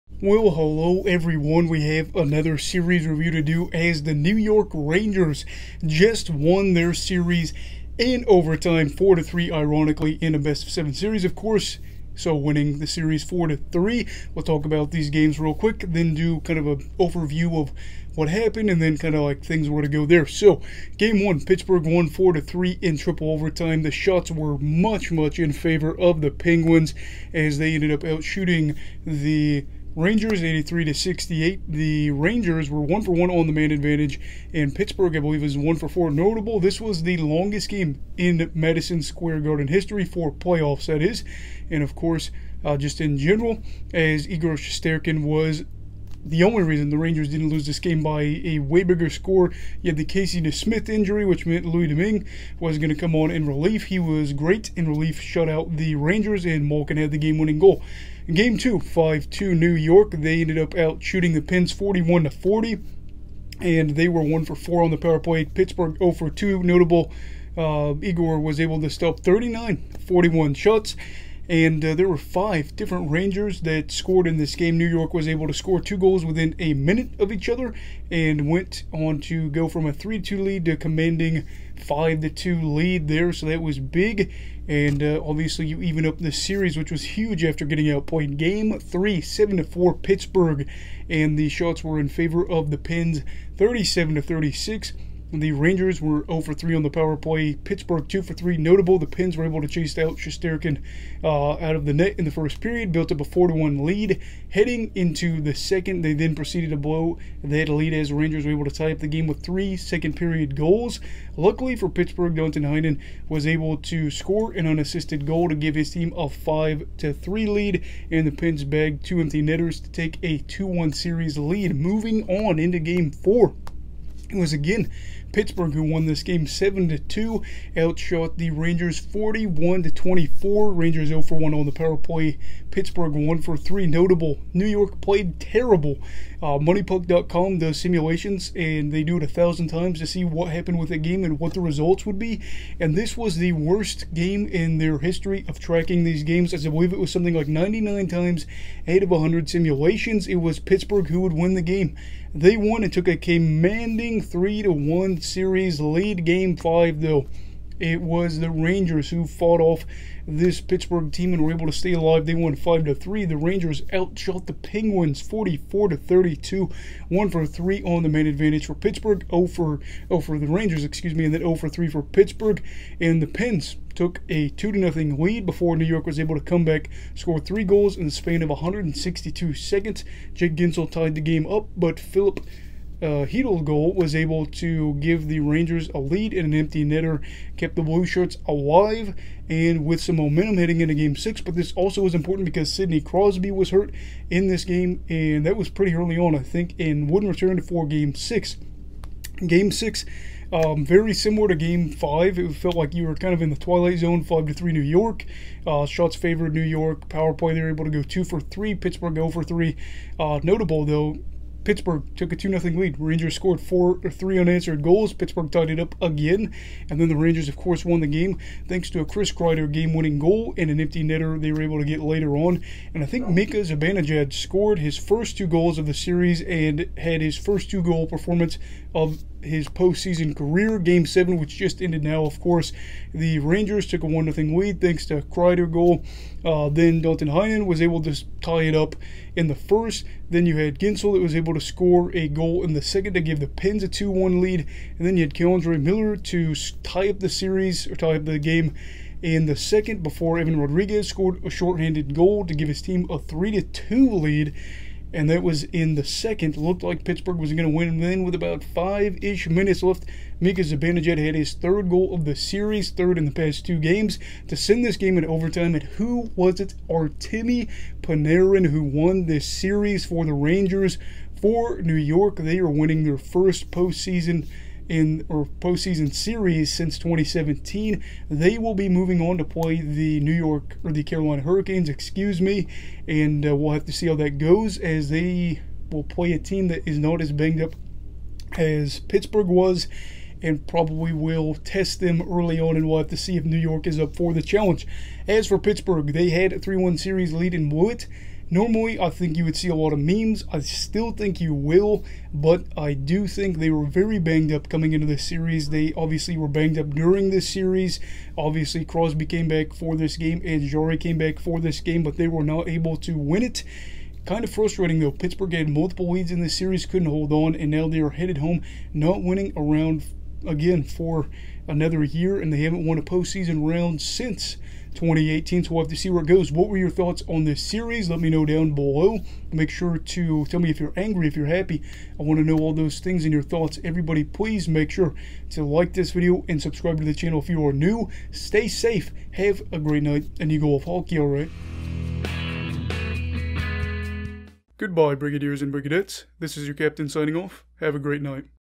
New York Rangers vs. Pittsburgh Penguins series review (2022 Stanley Cup Playoffs)
New York wins the series, as they come back from a 3-1 deficit!